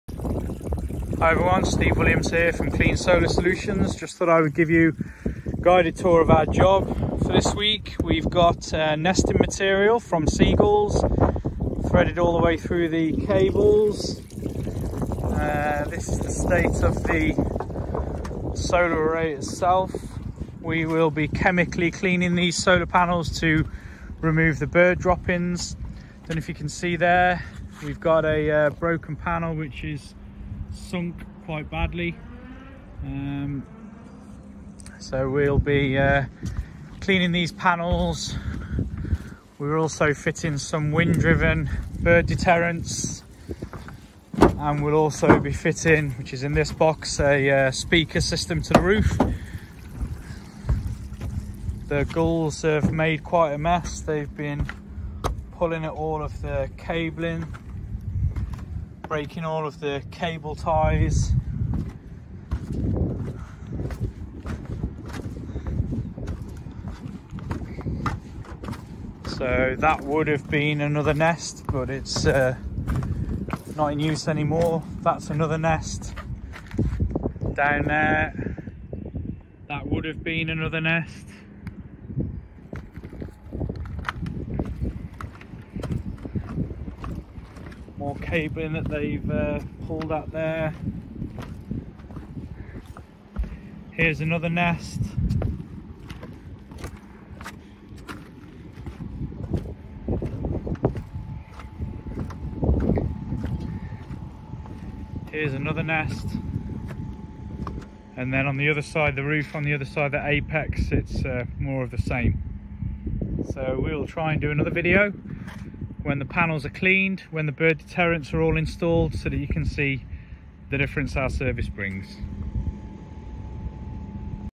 Effects Of Birds On Solar Panels - Part 1
Here we show the effects of birds, particularly gulls on solar panels.  We walk the first of two roofs infested with gulls to show the destruction they bring.

For information and purchasing of the bird deterrents seen here, please
Products available for global shipping.

Part 2 - Walking roof two
Part 3 - Chemically cleaning the panels
Part 4 - Walking the roof showing cleaned panels and the bird deterrents installed and working